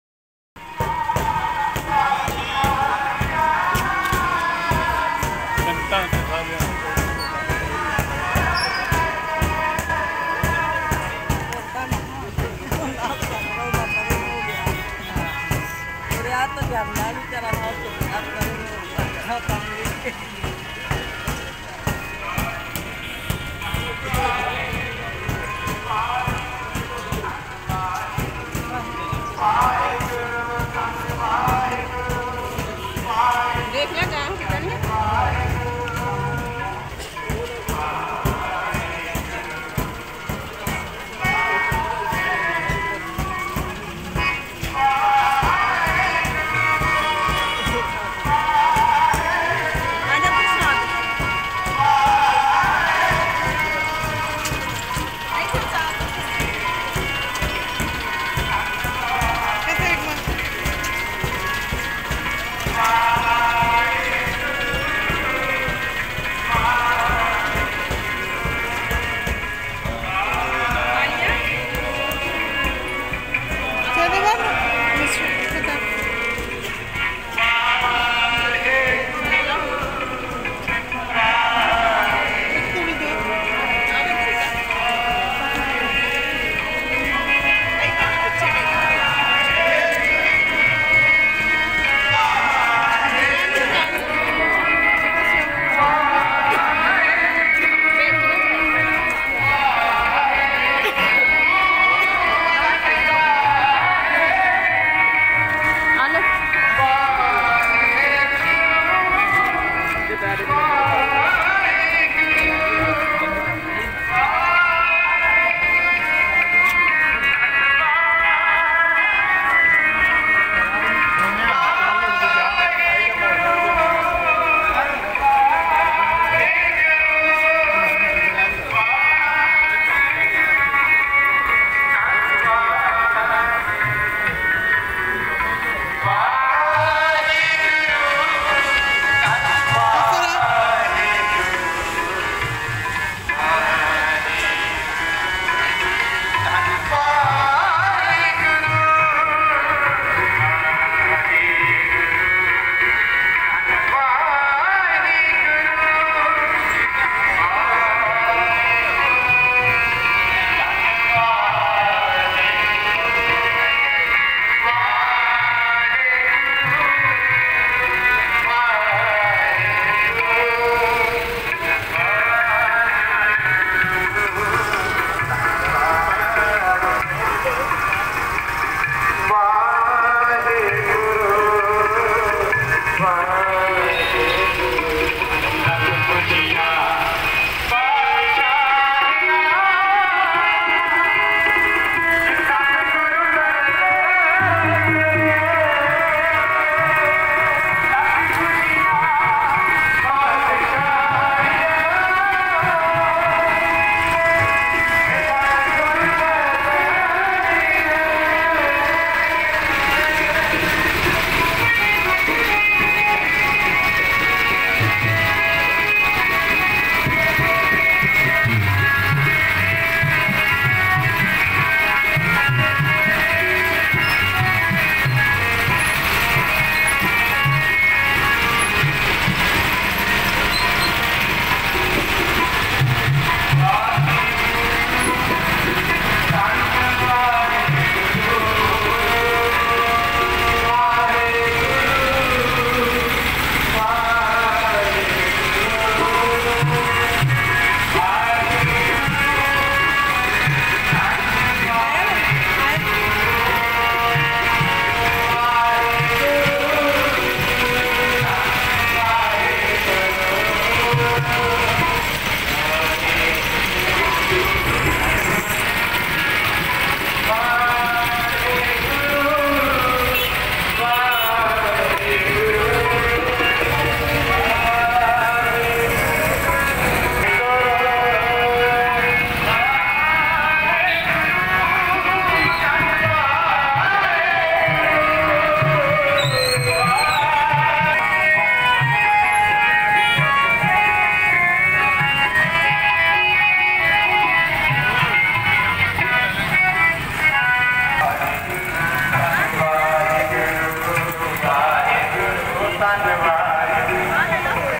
Nagar Kirtan at Makhu, Punjab, India
Nagar Kirtan du 7 novembre 2022. Kirtan est un terme utilisé dans le sikhisme pour désigner une procession de croyants qui chantent à travers les rues tout en portant le Guru Granth Sahib, le livre saint des sikhs, livre vénéré. Nagar Kirtan pourrait se traduire par: chants de prières pour le voisinage, ou, chants de prière pour la ville.